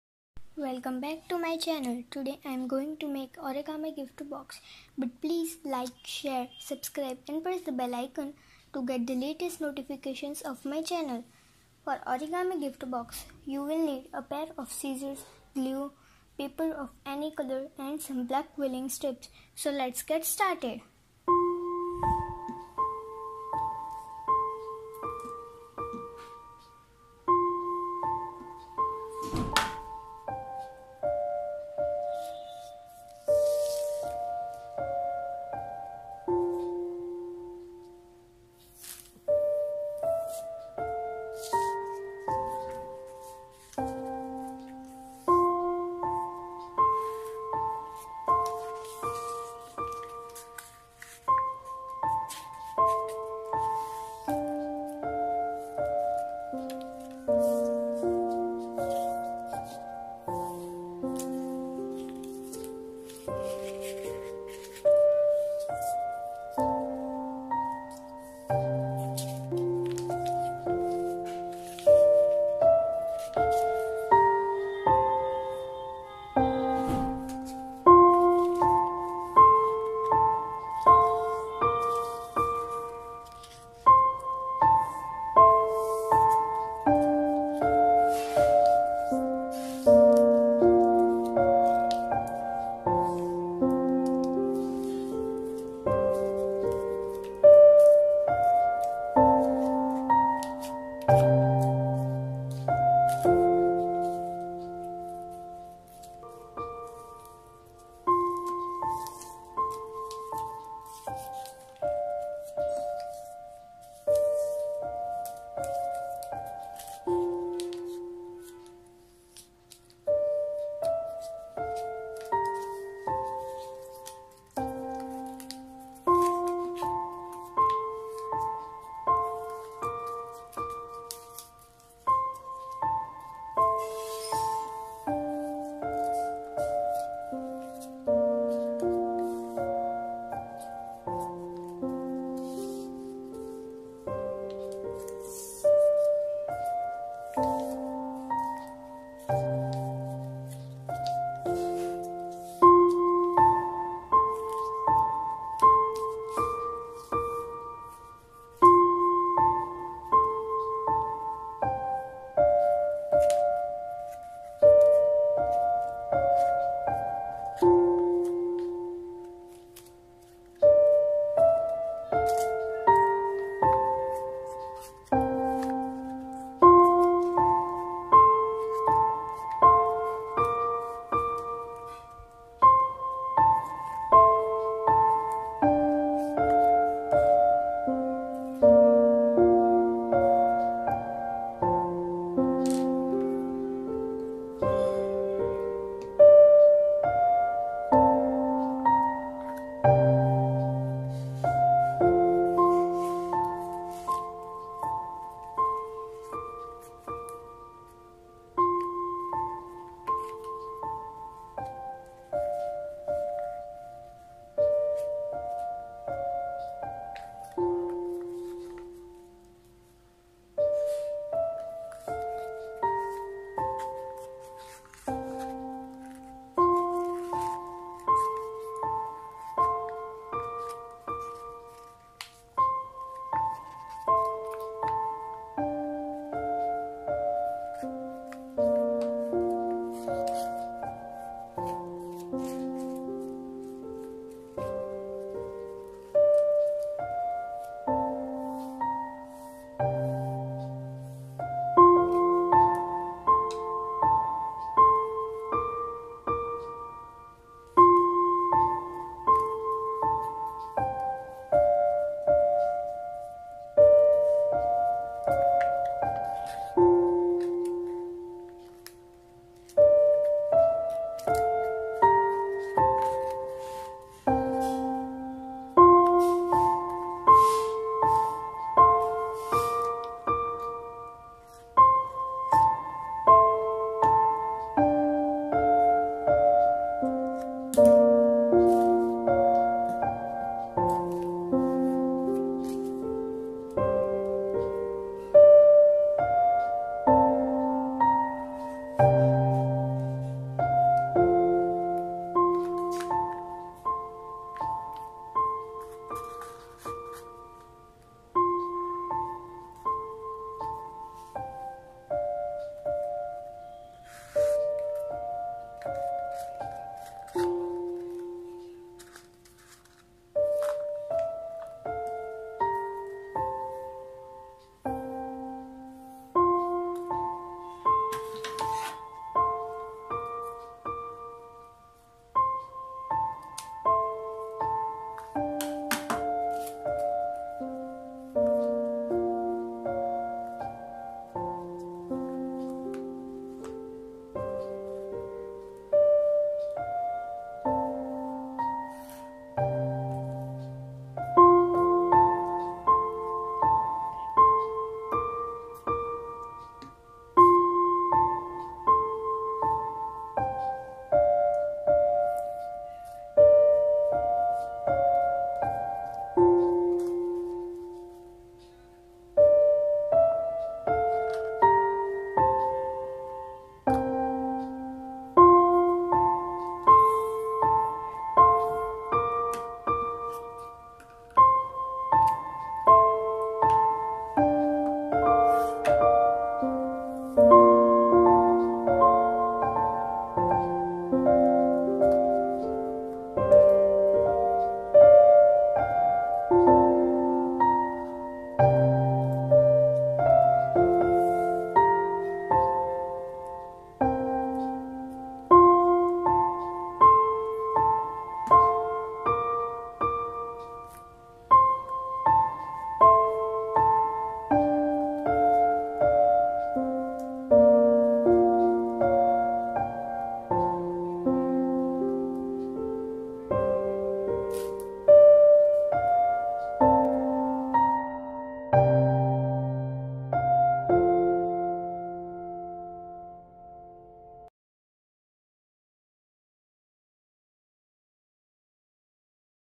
How to make origami gift box | DIY | EASY
In this video I have shown how to make origami gift box. It is very easy and you can try during lockdown.